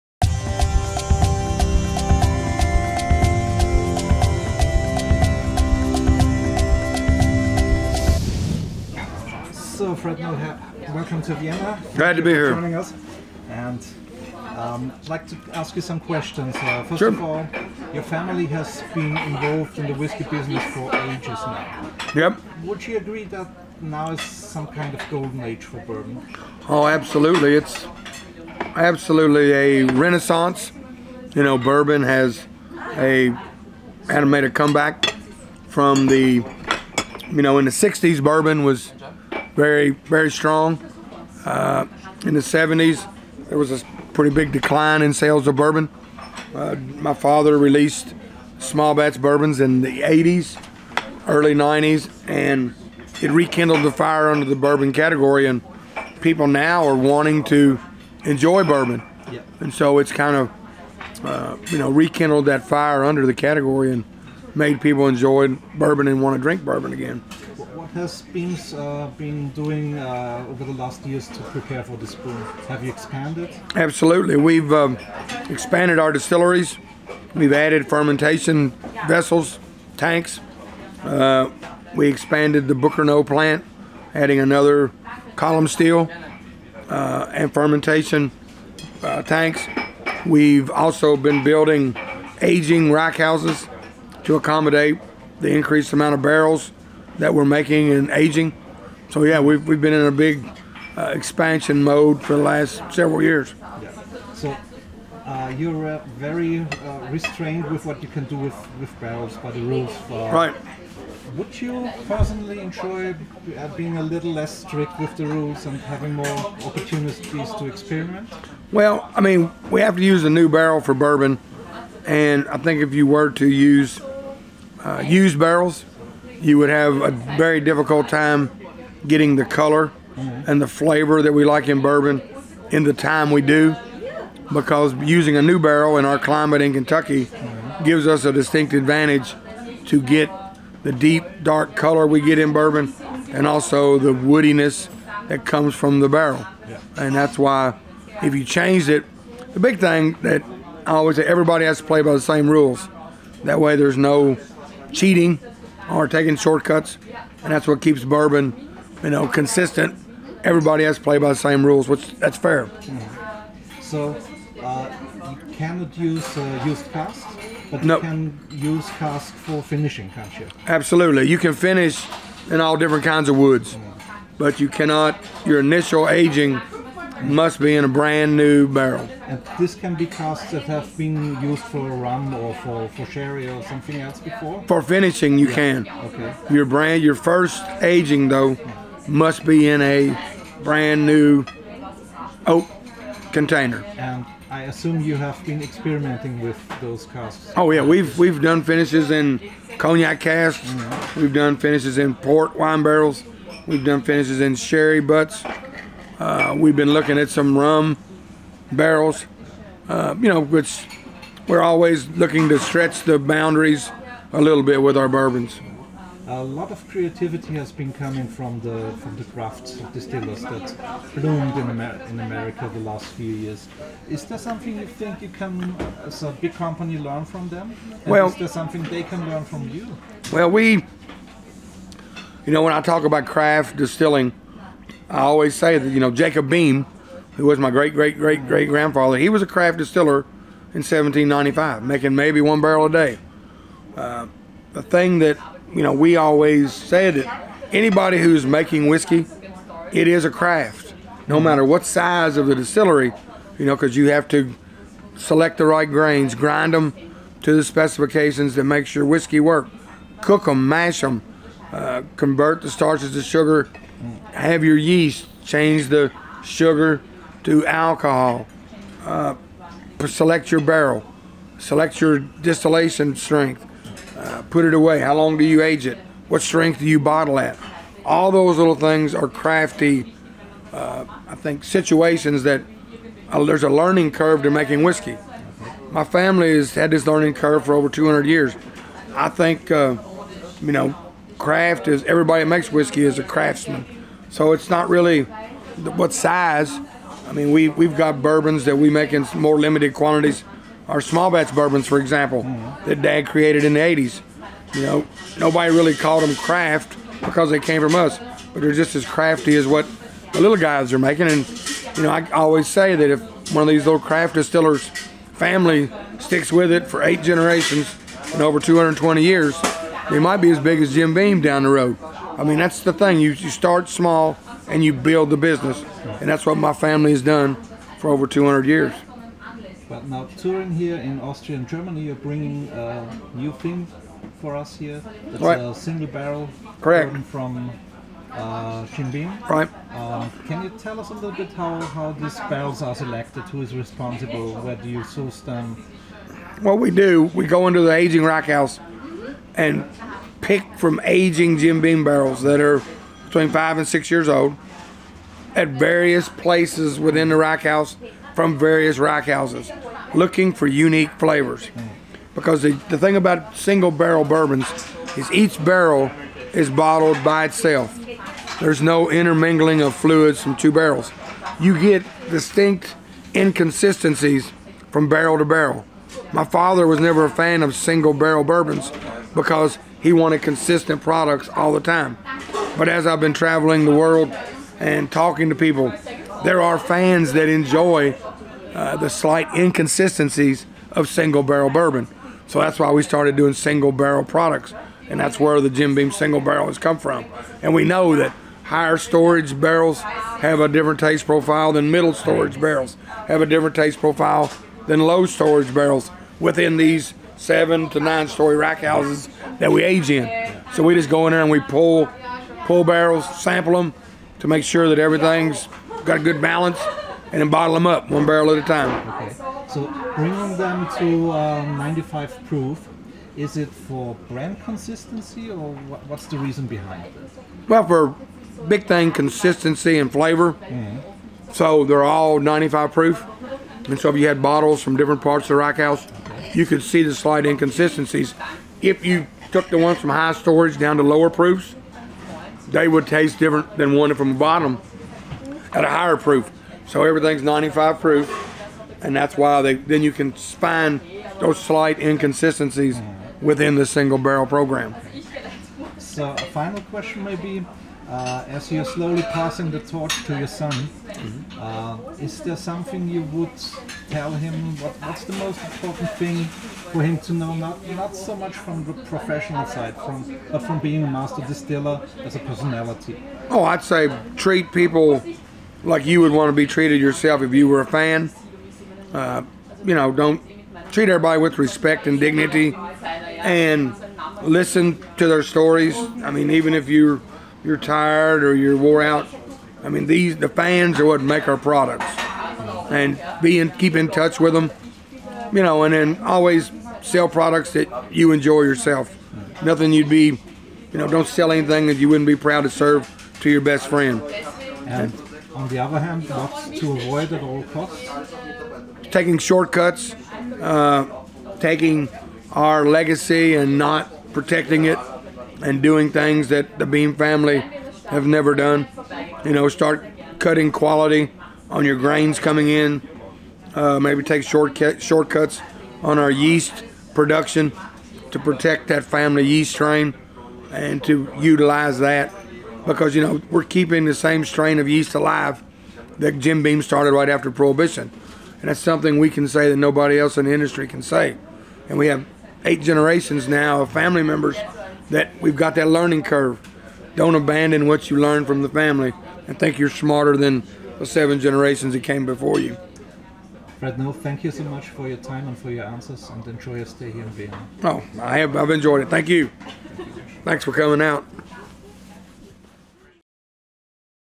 A conversation with Fred Noe, Jim Beam's Master Distiller
Fred Noe talks about the Bourbon Boom, Single Barrels, Experiments and Passing The Torch